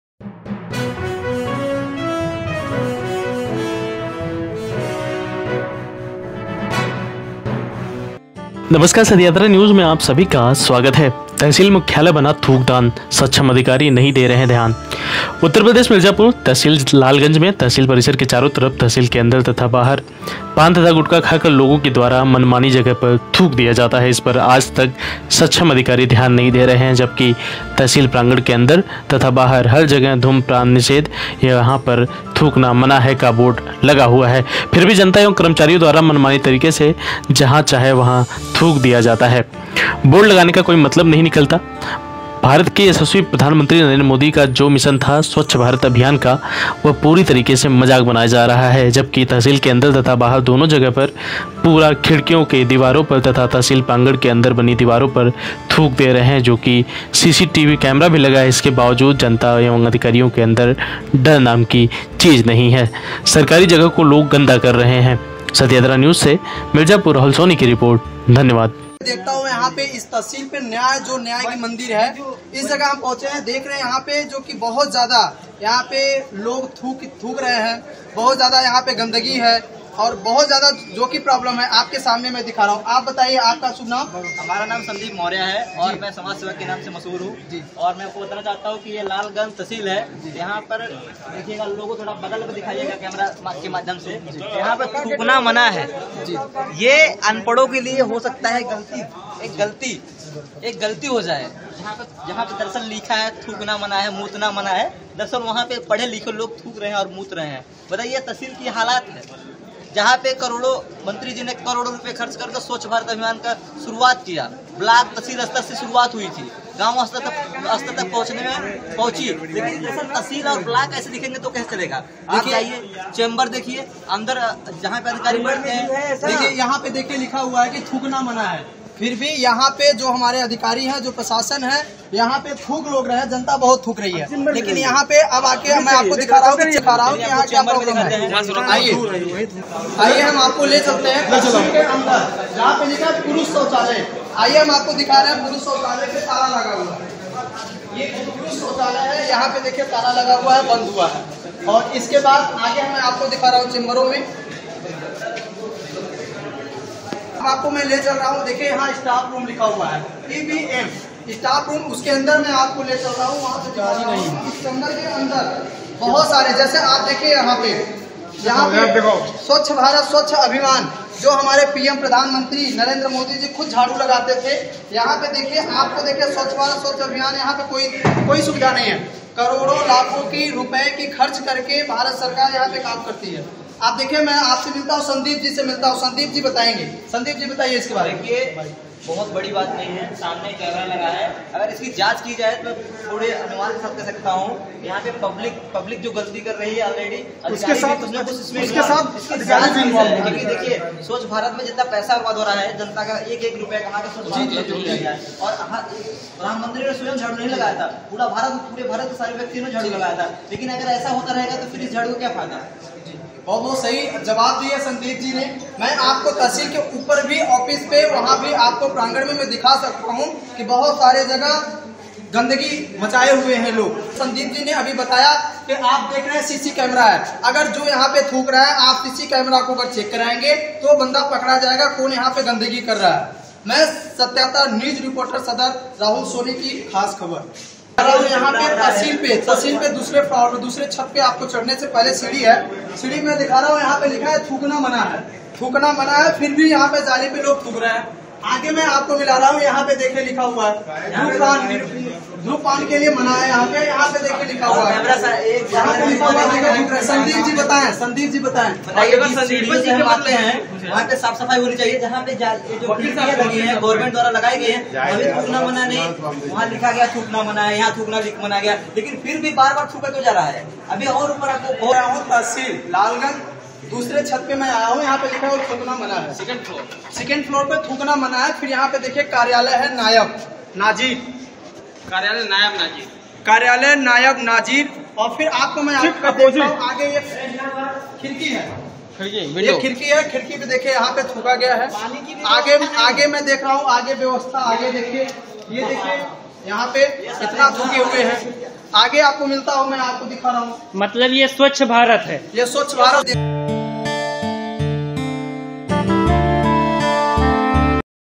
तहसील मुख्यालय बना थूकदान सक्षम अधिकारी नहीं दे रहे ध्यान
सत्य यात्रा न्यूज भारत सरकार द्वारा मान्यता प्राप्त साप्ताहिक अख़बार है।